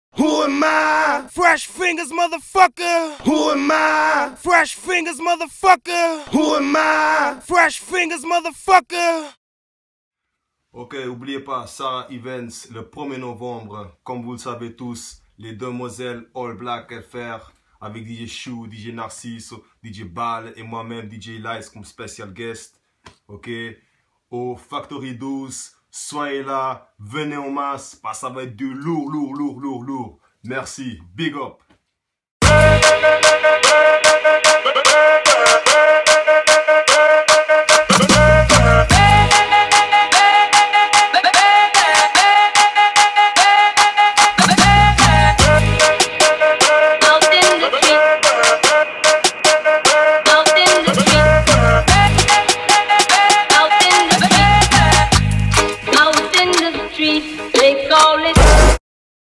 Promo Video Les Demoiselles " All Black Affair"
Promo Video Sarah Events
Tout le monde est obligé de s'habiller en noir!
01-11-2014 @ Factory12
Created by Dj Ilais